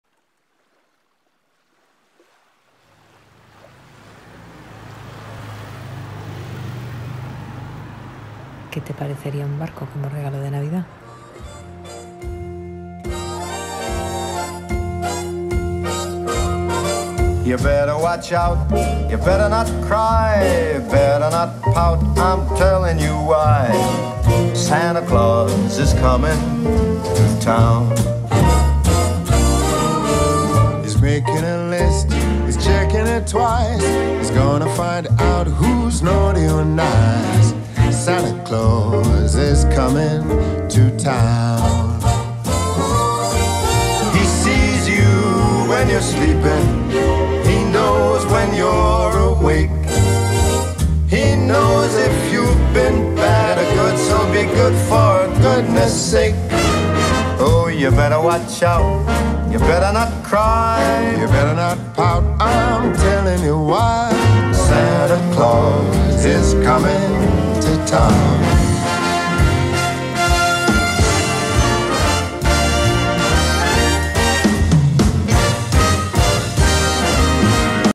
Un barco por Navidad
¿Qué te parecería un barco como regalo de Navidad?

En Just the Sea tenemos los barcos más bonitos y con más vidas que podrás encontrar. Se trata de los barcos realizados a partir de maderas de deriva. Realizados por Alberto Marco, artista que hace auténtica poesía de mar con sus manos.

Los barcos realizados por Alberto Marco, y que puedes encontrar dentro de nuestro espacio SEA LOVERS ART PROJECT, son barcos bellos, con detalles ingeniosos y únicos. Cada pieza inspira al autor un tipo de barco, velero, carguero arenero, pesquero, cada uno diferente en forma, color y espíritu. Todos cargados de vida con alma de mar.

Estos barcos son auténticas piezas de arte que llenan de vida y buena onda cualquier espacio donde sean expuestos.

Hay para todos los gustos. ¿Cuál es el tuyo?

La ballena que aparece en el vídeo duró poco tiempo en nuestro escaparate. Ahora está felizmente surcando otro mares. Le deseamos lo mejor, como a todas las piezas que están zarpando cada día con rumbos diversos.

¡Feliz travesía a todos!

Puedes encontrar toda la información sobre el origen de las maderas y los barcos